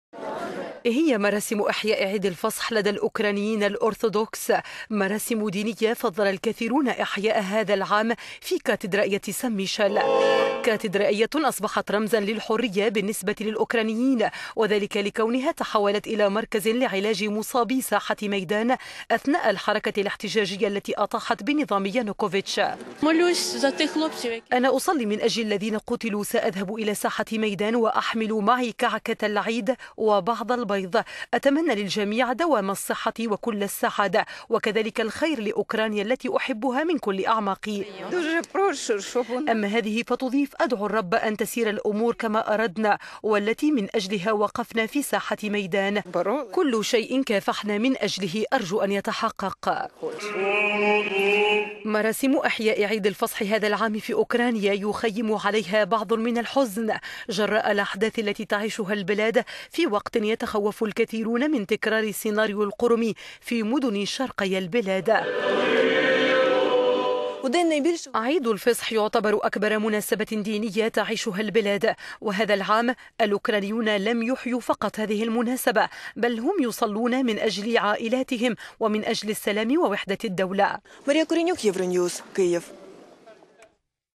أوكرانيا: الخوف من تقسيم البلاد يخيم على مراسم إحياء عيد الفصح
هي مراسم إحياء عيد الفصح لدى الأوكرانيين الأورثودكس. مراسم دينية فضل الكثيرون إحياءها هذا العام في كاتيدرائية سان ميشال. كاتيدرائية أصبحت رمزا للحرية بالنسبة للأوكرانيين وذلك لكونها تحولت إلى مركز لعلاج مصابي ساحة ميدان أثناء الحركة الإحتجاجية التي أطاحت بنظام يانوكوفيتش.

تقول إمرأة:"أنا أصلي من أجل الذين قتلوا، سأذهب الى ساحة ميدان وأحمل معي كعكة العيد وبعض البيض. أتمنى للجميع دوام الصحة وكل السعادة. وكذلك الخير لأوكرانيا التي أحبها ".
تضيف أخرى:" أدعو الرب أن تسير الأمور كما أردنا والتي من أ...